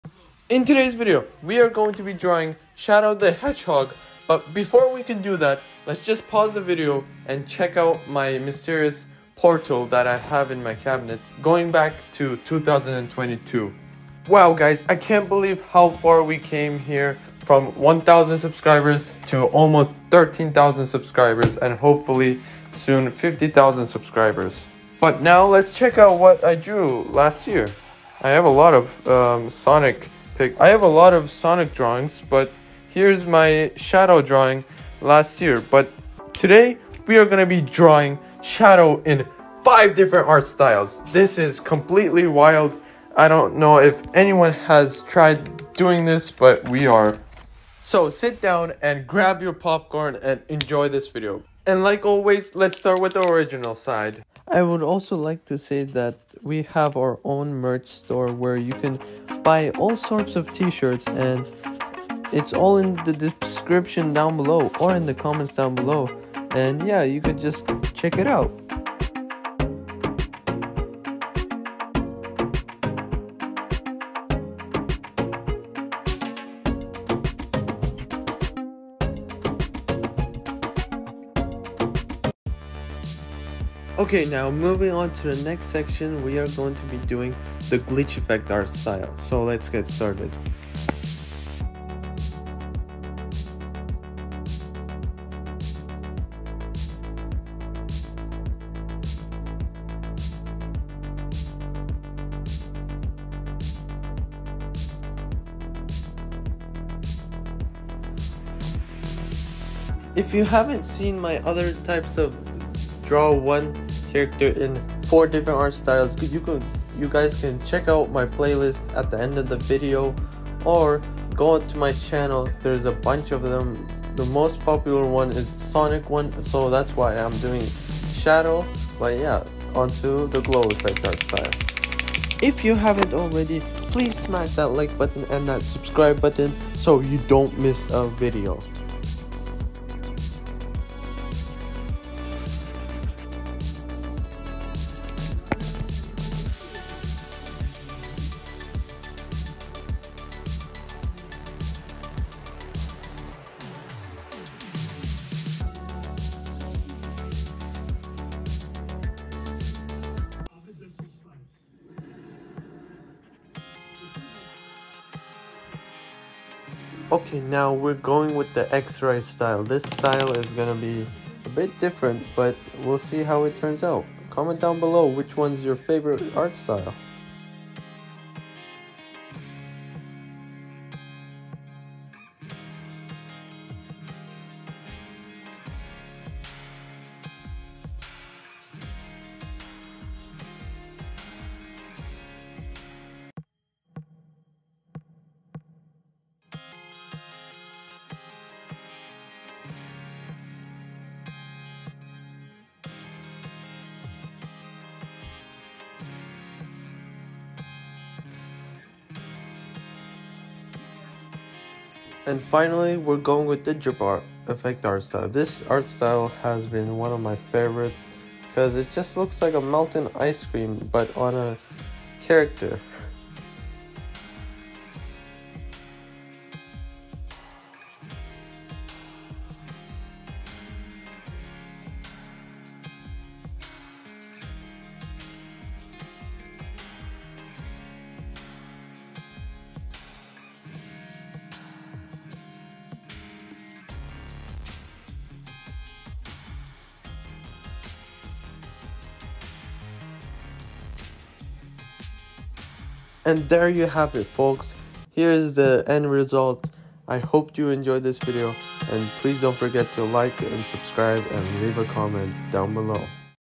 🔥🤩 One Drawing, BUT IN 5 DIFFERENT Art Styles!
Watch as we take on the challenge of drawing Shadow in not one, not two, but 5 different art styles! This video is packed with artistic variety, from the original design to pixel art and even a stunning realism rendition. Stay fresh and enjoy the video!

Time Stamps
0:00 - 0:31 Intro
1:30 Original
2:09 Glitch
2:49 Glow/ Neon
3:33 X-Ray
4:24 Drip